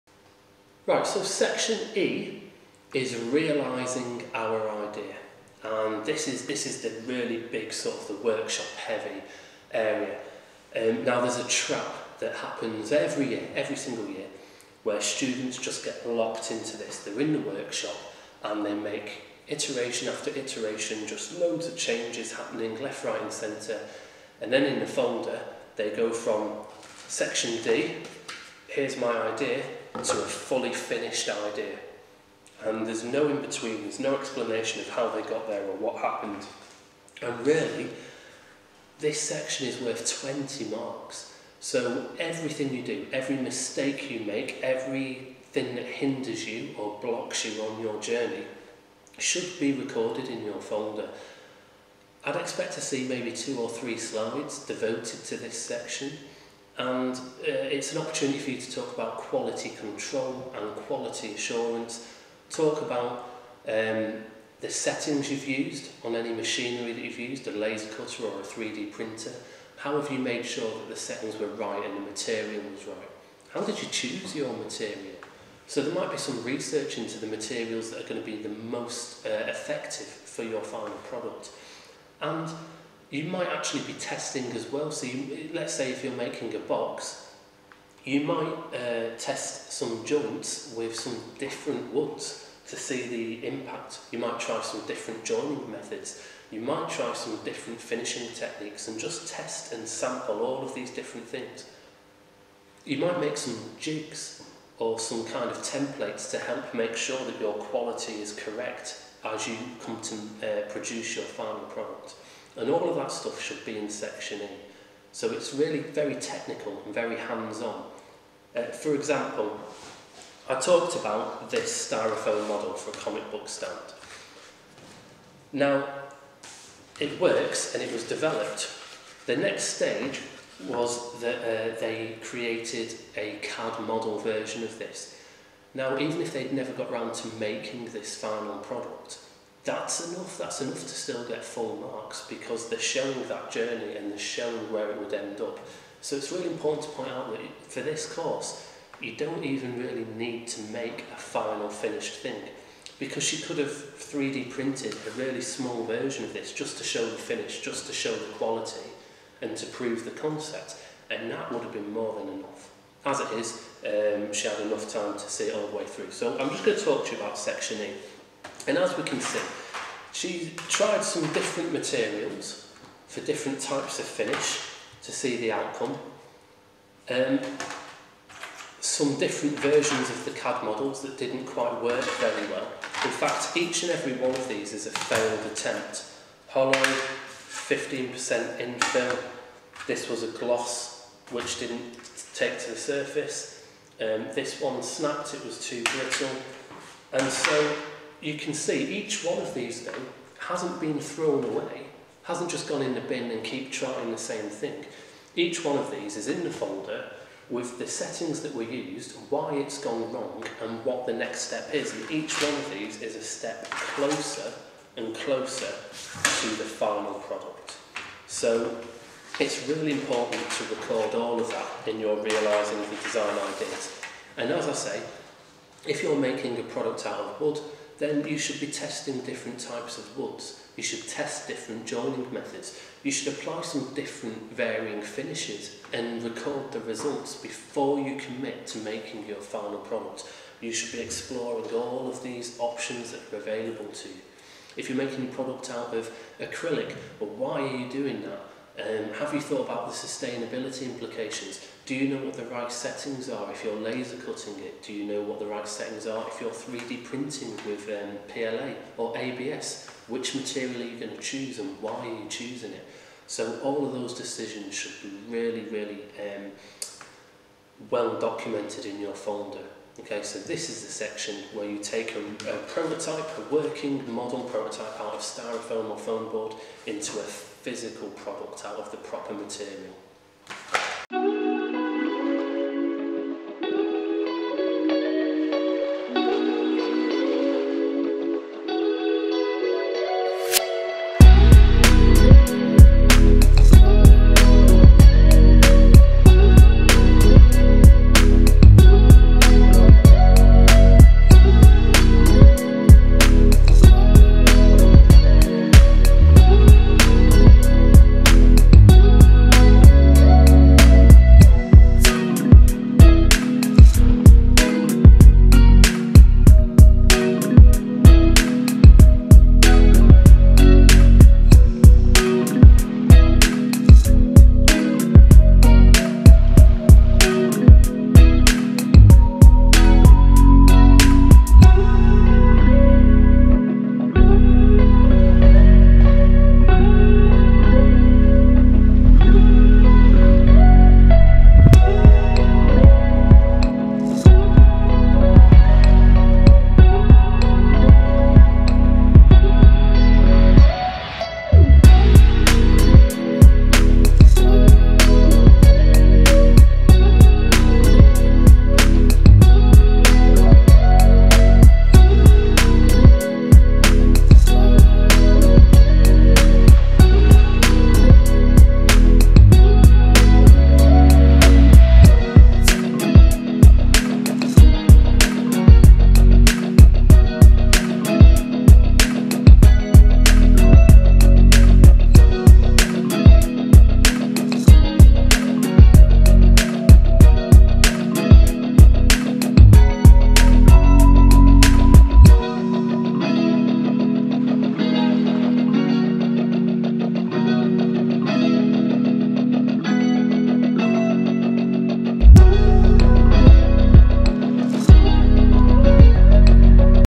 Section E Design Technology GCSE NEA
An overview of what things should be included in Section E realising design ideas for the GCSE DT NEA.
NOTE- for 2021 the marks awarded for this section will be reduced.